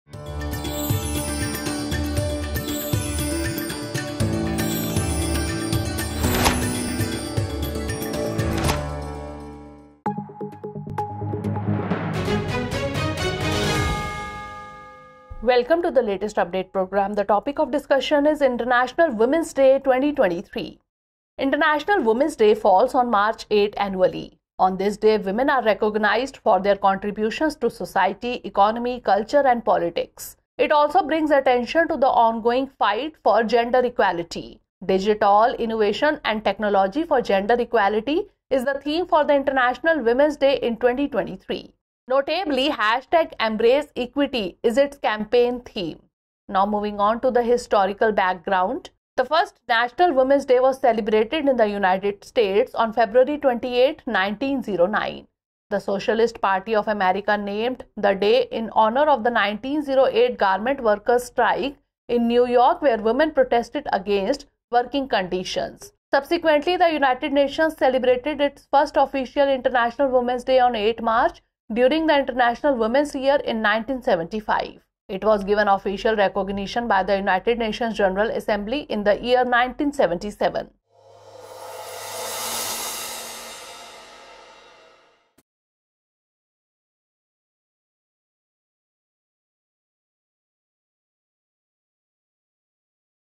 International Women's Day 2023: Latest update | Drishti IAS English
International Women's Day falls on March 8 annually. ‘DigitALL: Innovation and technology for gender equality’ is the theme for International Women's Day in 2023. EmbraceEquity is its campaign theme.